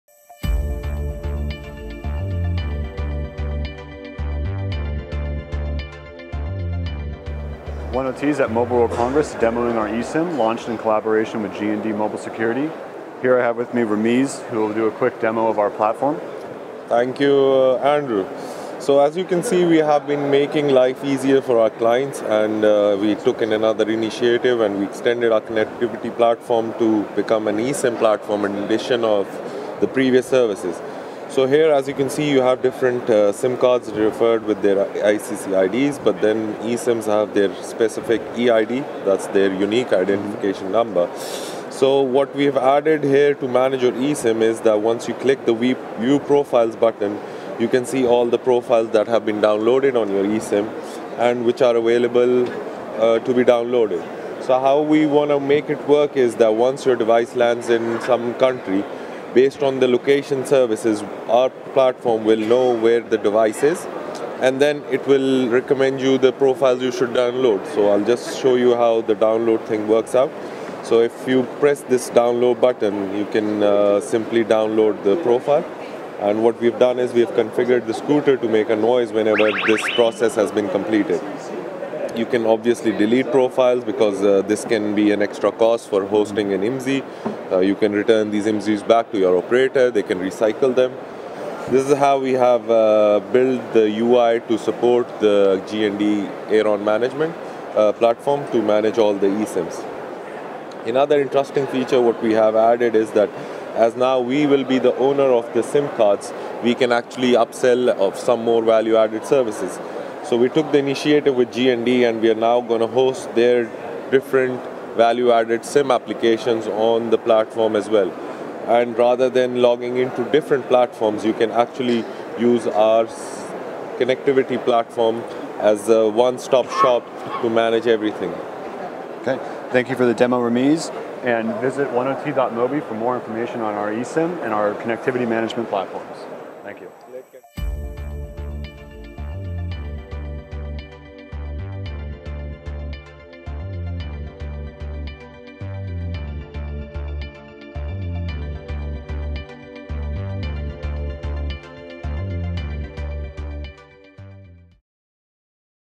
1oT eSIM Video Series #1: 1oT eSIM Demo at Mobile World Congress 2019
1oT is a carrier independent connectivity provider for global IoT service providers.

We launched the eSIM for IoT at Mobile World Congress 2019. The revolutionary solution was pushed to the market in collaboration with Giesecke+Devrient Mobile Security the world’s leading eSIM management service provider. Technically known as eUICC, eSIM is an unlocked SIM card that enables the client to manage different carrier services “over-the-air” without the need to physically change the SIM card. Meaning, today you might use Vodafone services, but tomorrow decide to switch to AT&T without touching the physical SIM card itself.

In 1oT eSIM Video Series #1 we are demoing 1oT eSIM and show how it works with 1oT Terminal (our Connectivity Management Platform).